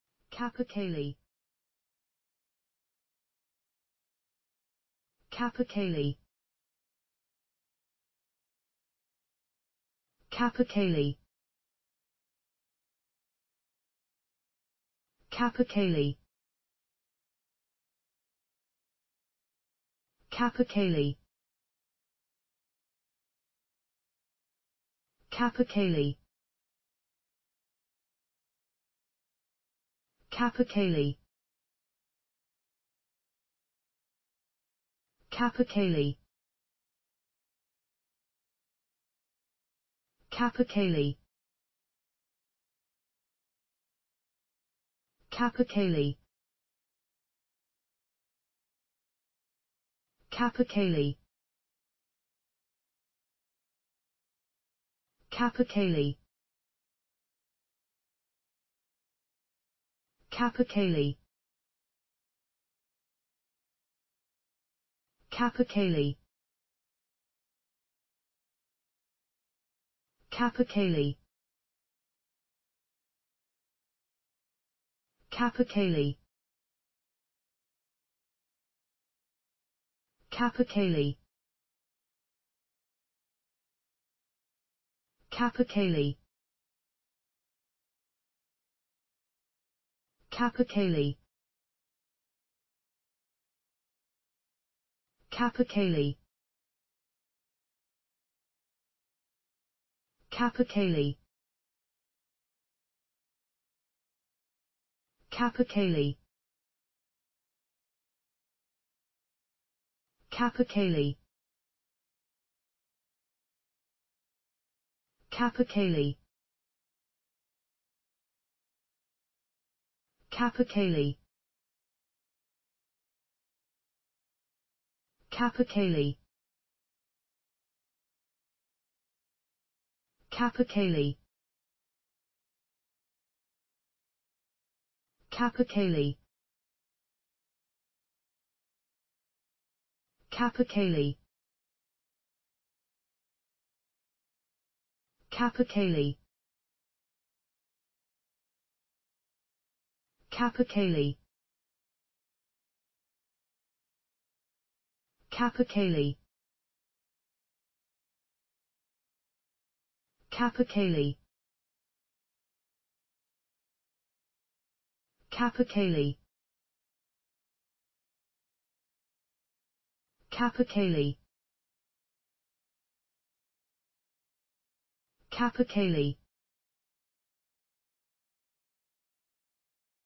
capercaillie. How to pronounce the english word capercaillie .Start with C. Learn from me.
Follow me!Learn from me how to pronounce the english word capercaillie.This video shows you how to pronounce the English word capercaillie .